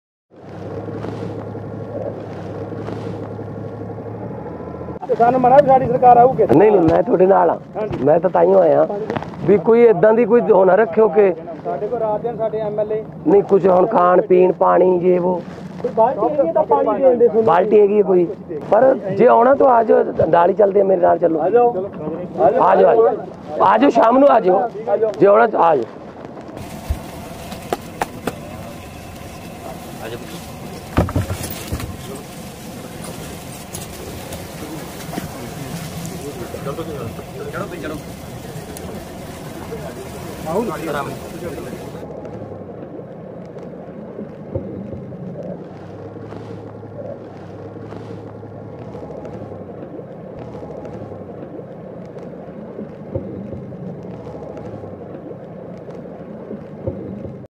Punjab Floods માં ફસેલાં પરિવારને બચાવવા પહોંચ્યા CM Bhagwant Mann👏| સર્વત્ર પ્રશંસા મળી રહી છે
Punjab Floods માં ફસેલાં પરિવારને બચાવવા પહોંચ્યા CM Bhagwant Mann 👏| સર્વત્ર પ્રશંસા મળી રહી છે | AAP Punjab | Aam Aadmi Party Gujarat